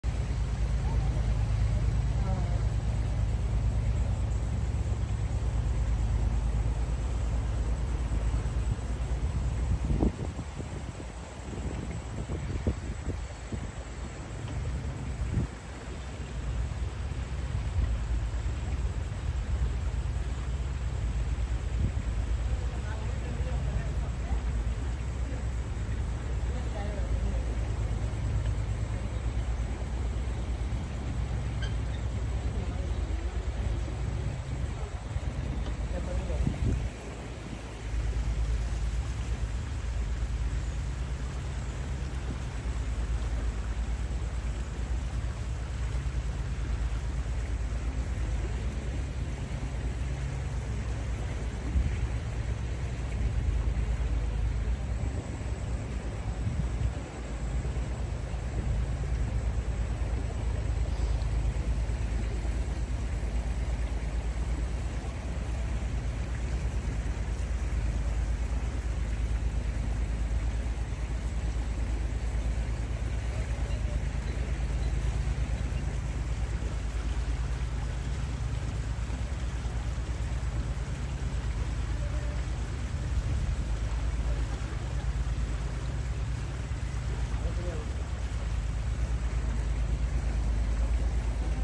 Backwater House Boat ride Kumarakom Kerala - fun times
Backwaters House Boat ride in Kumarakom Kerala India - ride in Allapey lake with food and amazing music
fun times - Vacation Kerala